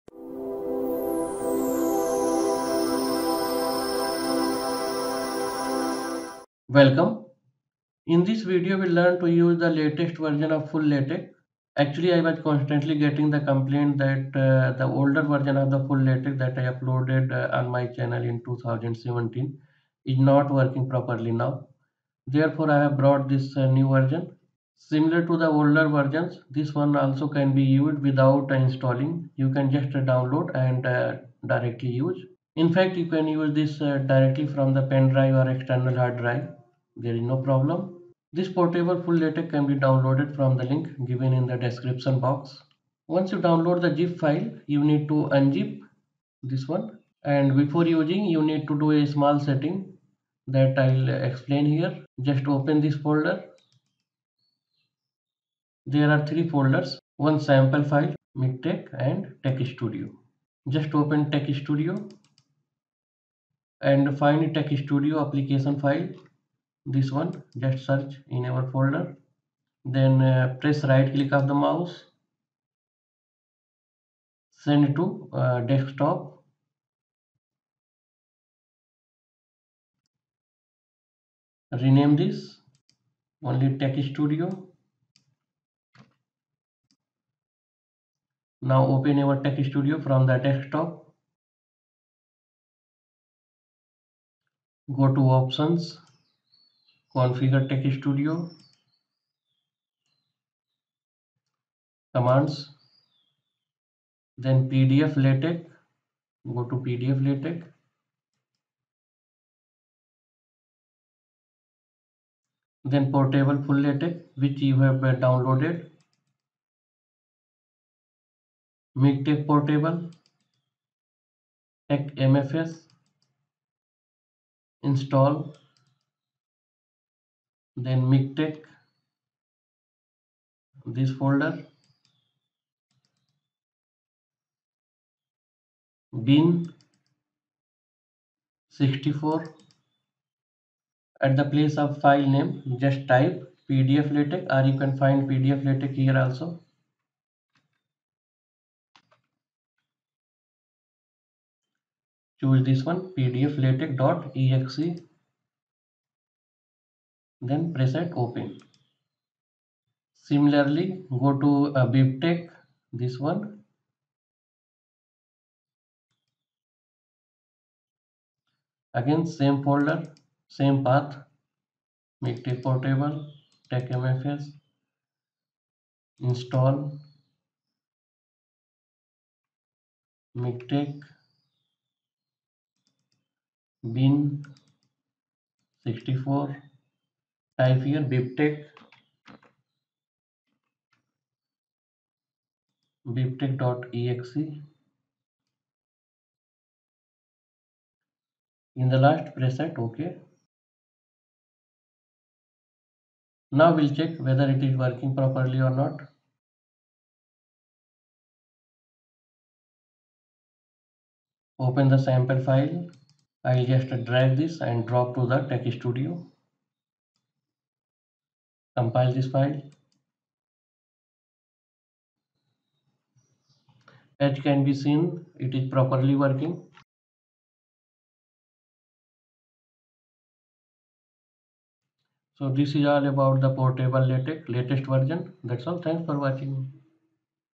Latest Portable LaTeX 2023: MikTeX and TeXstudio (Latex Advanced Tutorial-37)
The easiest way to use the latest LaTeX (MikTeX + TeXstudio) without installing the software.  This software can be used on a computer or can be run from an external hard disk/USB pen drive/sd card.

Compiler's path you need to set into your TeXstudio setting:
Compiler path: ...install/texmfs/miktex/bin